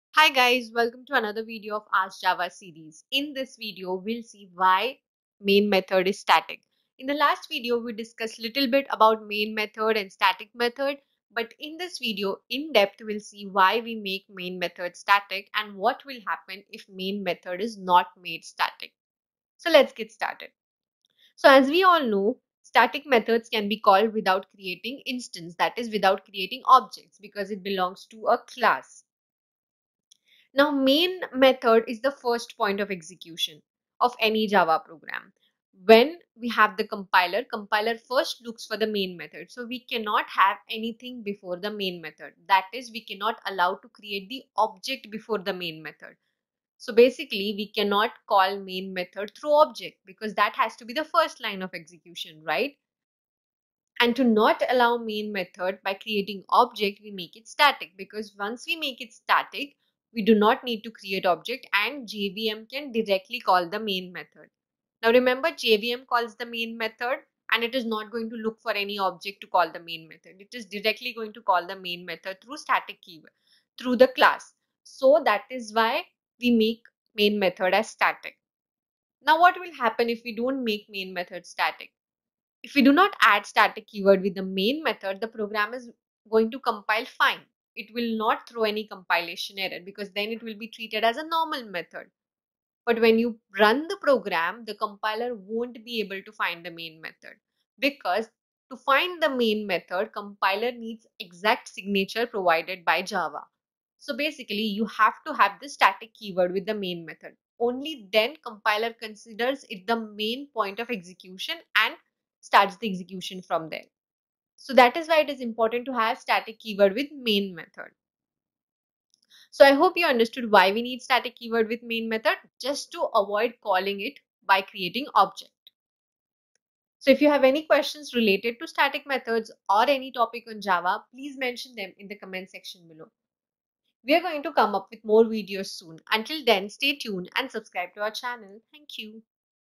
why is main method static in java? | Java Interview Questions and Answers | Ask Java | Code Bode
Hi Guyz,
In this video, we will see why do we need to make the main method static in java and what will happen if the main method is not static.